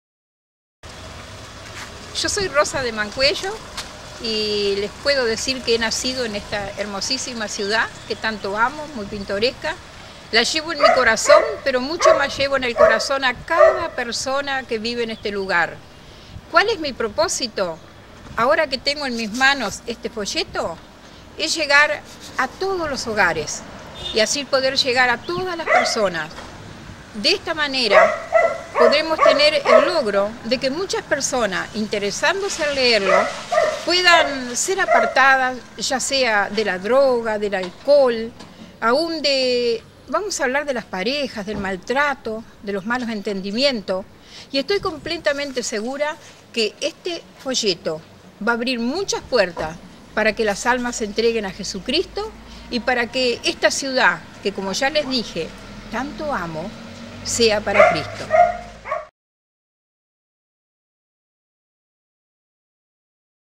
ROSA de MANCUELLO con una pasión auténtica en AIGUA por CADA HOGAR
En esta pequeña ciudad, por muchos años sembrando el evangelio junto a su esposo Cristobal Mancuello, ahora Rosa debe hacerlo JUNTO A SU CONGRECACION.  Su pasión por los perdidos y el buen testimonio en la ciudad, hace que la gente reciba CADA FOLLETO de "LA PALABRA A CADA HOGAR" con autoridad en si misma, por los años de testimonio y siembra del mensaje de Salvación. ¡¡Su pasión por llevar las nuevas se nota a cada minuto caminando por las calles de Aiguá!!